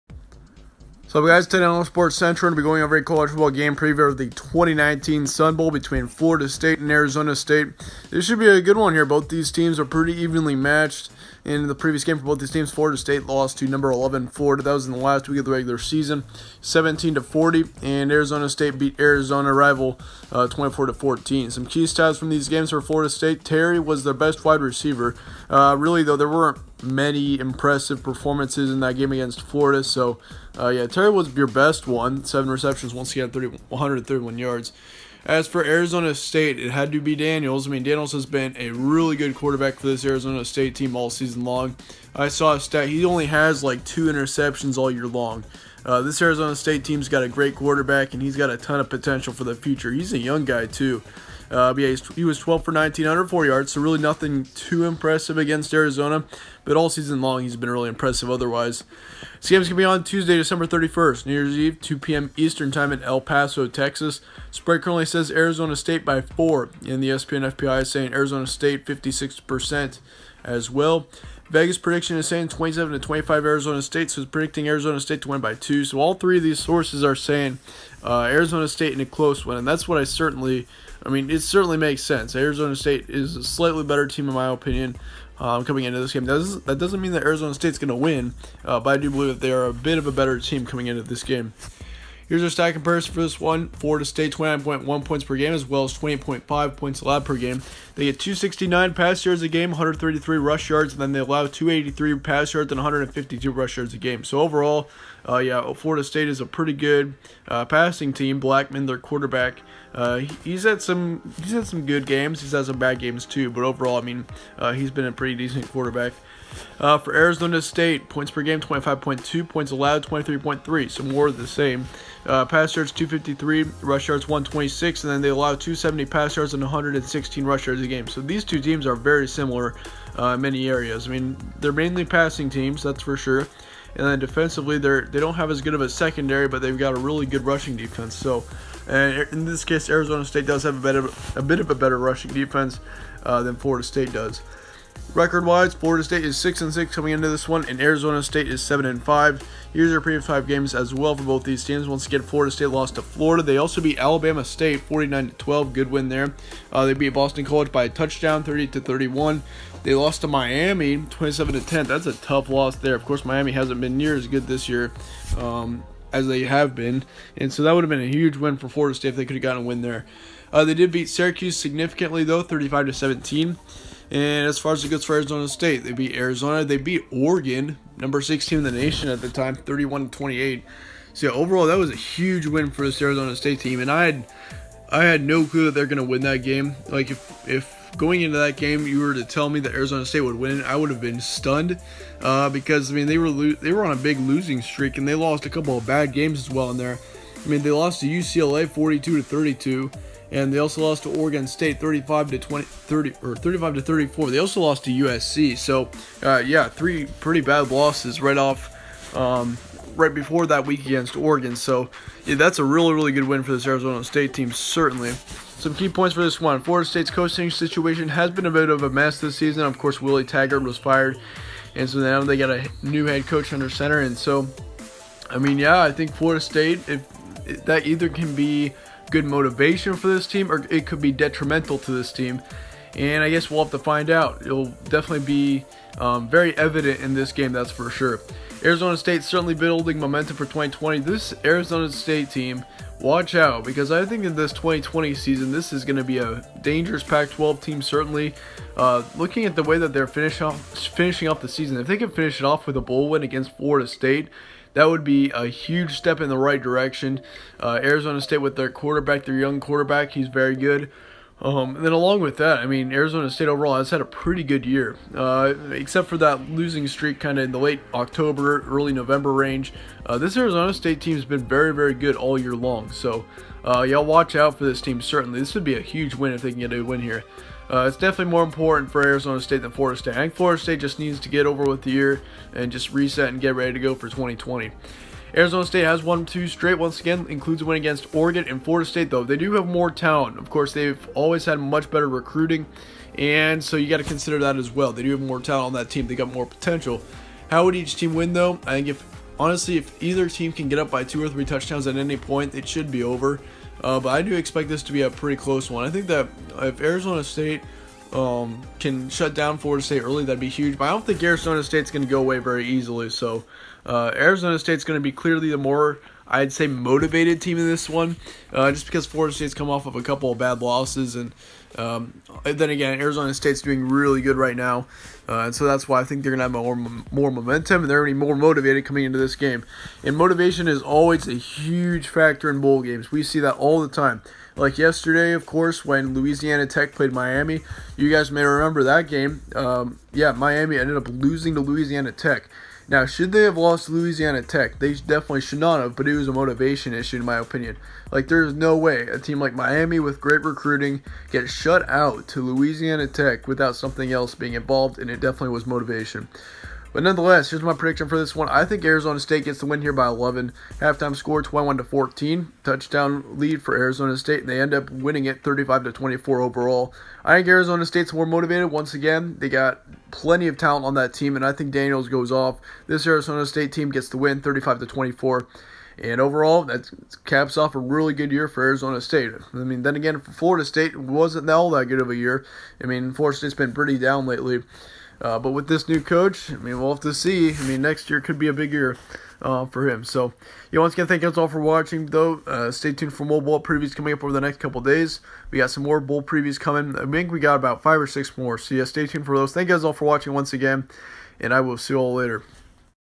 Florida State vs Arizona State Preview and Predictions- 2019 Sun Bowl
Today we go over a preview on the sun bowl.